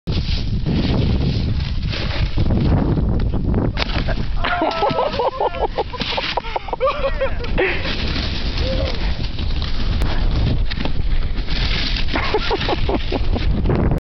Adam's infamous wipeout above St. Mary's.
title says it all.  In early October, we found a booter while hiking way above St. Mary's Glacier, so we decided to hit it.